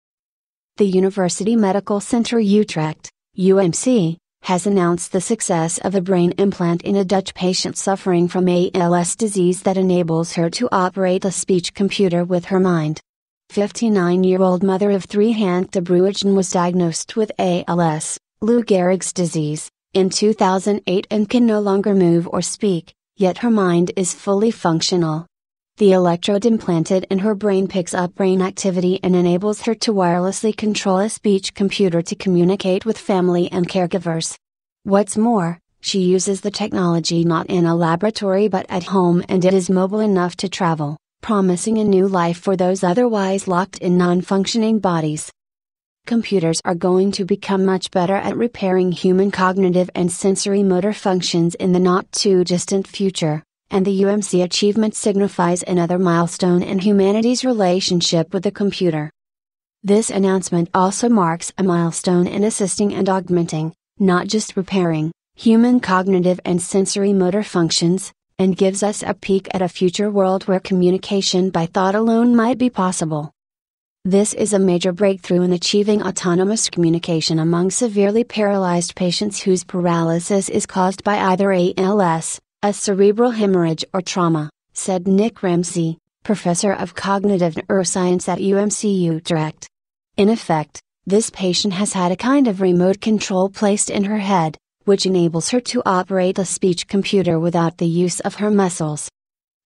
Brain implant enables paralyzed woman to communicate by thought
The University Medical Center Utrecht (UMC) has announced the success of a brain implant in a Dutch patient suffering from ALS disease that enables her to operate a speech computer with her mind. Fifty-nine year-old mother-of-three Hanneke de Bruijne was diagnosed with ALS (Lou Gehrig's disease) in 2008 and can no longer move or speak, yet her mind is fully functional. The electrode implanted in her brain picks up brain activity and enables her to wirelessly control a speech computer to communicate with family and caregivers. What's more, she uses the technology not in a laboratory but at home and it is mobile enough to travel, promising a new life for those otherwise locked in non-functioning bodies.

Computers are going to become much better at repairing human cognitive and sensory-motor functions in the not-too-distant future, and the UMC achievement signifies another milestone in humanity's relationship with the computer.

This announcement also marks a milestone in assisting and augmenting (not just repairing) human cognitive and sensory-motor functions, and gives us a peak at a future world where communication by thought alone might be possible.

This is a major breakthrough in achieving autonomous communication among severely paralyzed patients whose paralysis is caused by either ALS, a cerebral hemorrhage or trauma," said Nick Ramsey, Professor of Cognitive Neuroscience at UMC Utrecht. "In effect, this patient has had a kind of remote control placed in her head, which enables her to operate a speech computer without the use of her muscles.